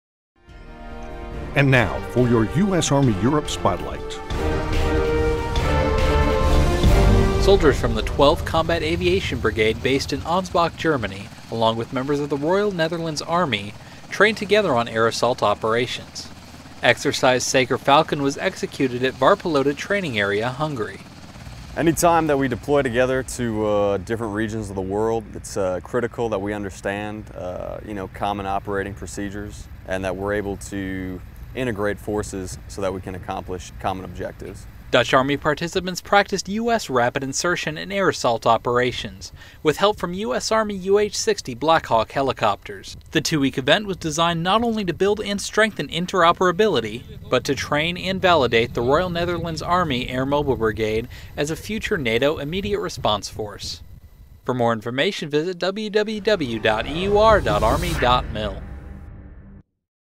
Saker Falcon 2014 Spotlight
SOLDIERS FROM 12TH COMBAT AVIATION BRIGADE, BASED IN ANSBACH, GERMANY, ALONG WITH MEMBERS OF THE ROYAL NETHERLANDS ARMY, TRAINED TOGETHER ON AIR ASSAULT OPERATIONS AT EXERCISE SAKER FALCON IN VARPALOTA TRAINING AREA, HUNGARY.